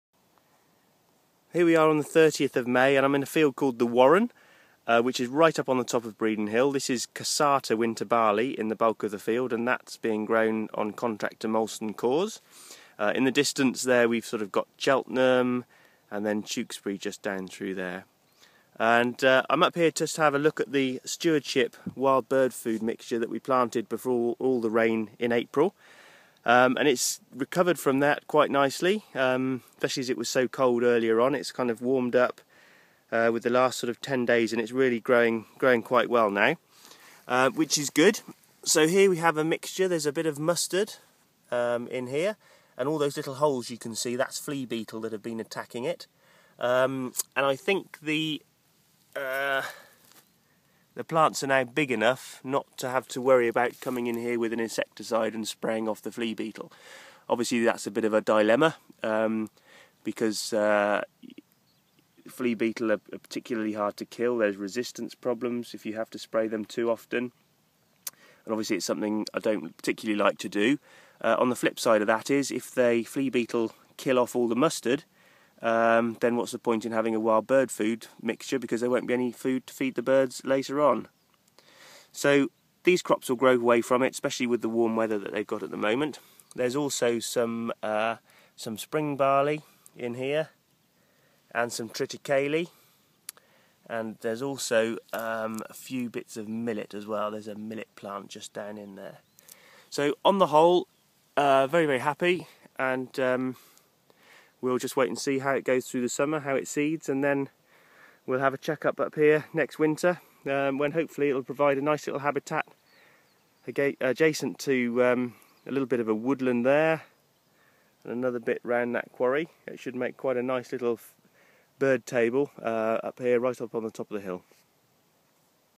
Emerging Bird Food
A stewardship mixture of spring barley, triticale, mustard and millet emerging from the cold wet April